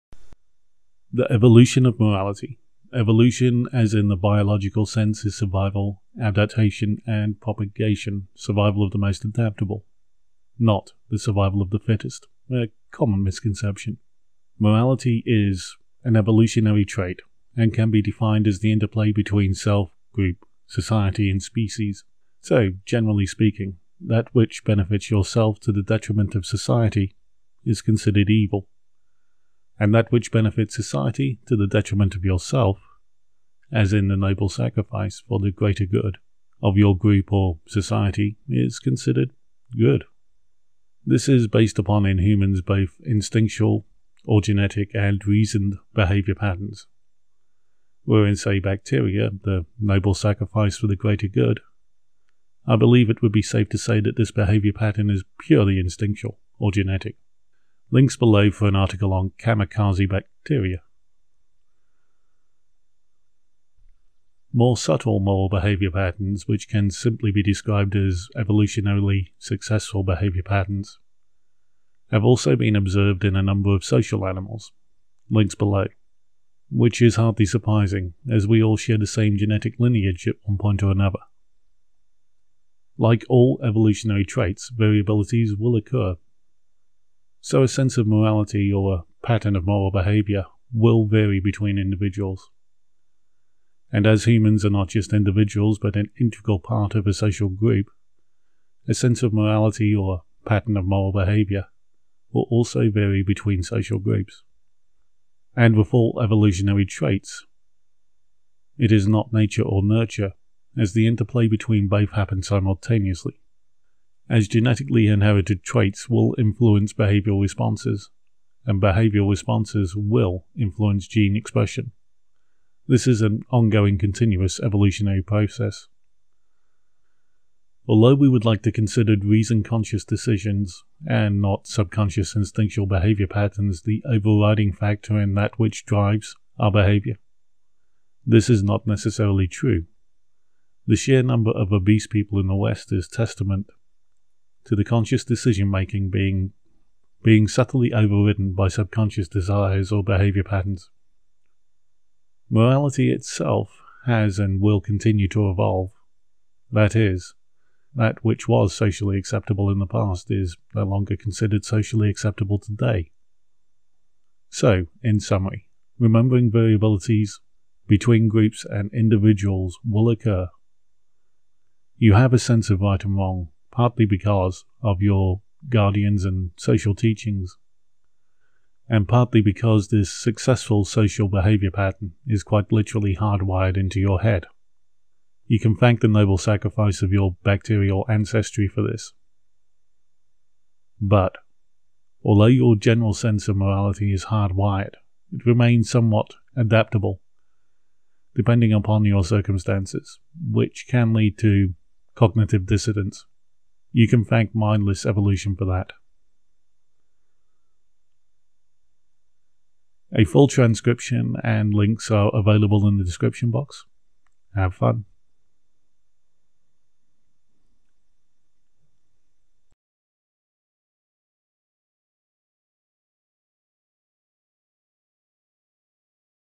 The evolution of morality
Evolution as in the biological sense is survival adaptation and propagation, survival of the most adaptable, not the survival of the fittest ( a common misconception )

Morality is a evolutionary trait, and can be defined as the interplay between self group society and species

So generally speaking that which benefits yourself to the detriment of society, is considered evil, and that which benefits society to the detriment of yourself as in the noble sacrifice for the greater good of your group or society is considered good

This is based upon in humans both instinctual or genetic and reasoned behaviour patterns

Where say in bacteria, the noble sacrifice for the greater good, I believe it would be safe to say that this behaviour pattern is purely instinctual or genetic ( link below for a article on kamikaze bacteria )

More subtle moral behaviour patterns, which can simply be described as evolutionarily successful behaviour patterns, have also been observed in a number of social mammals,(link below) which is hardly surprising as we all share the same genetic lineage at one point or another

Like all evolutionary traits variability's will occur, so a sense of morality or a patten of moral behaviour will vary between individuals, and as humans are not just individuals but a integral part of a social group a sense of morality or a patten of moral behaviour  will also vary between social groups

And with all evolutionary traits  it is not nature or nurture, as the interplay between both happens simultaneously

As genetically inherited traits will influence behavioural responses, and behavioural responses will influence gene expression, this is an ongoing continues evolutionary process

Although we would like to consider reasoned conscious decisions and not subconscious instinctual behaviour pattens the overriding factor in that which drives our behaviour, this is not necessarily true

The sheer number of obese people in the west is testament to the conscious decision making being subtly overridden by subconscious desires or behaviour pattens

Morality itself has and will continue to evolve, ie that which was socially acceptable in the past is no longer considered socially acceptable today

So in summary remembering variability between groups and individuals will occur, you have a sense of right from wrong partly because of your guardians and social teachings, and partly because this successful social behaviour pattern is quite literally hard wired into your head,  you can thank the noble sacrifice of your bacterial ancestry for this
But although your general sense of morality is hard wired it remains somewhat adaptable depending upon your circumstances which can lead to cognitive dissidents
You can thank mindless evolution for that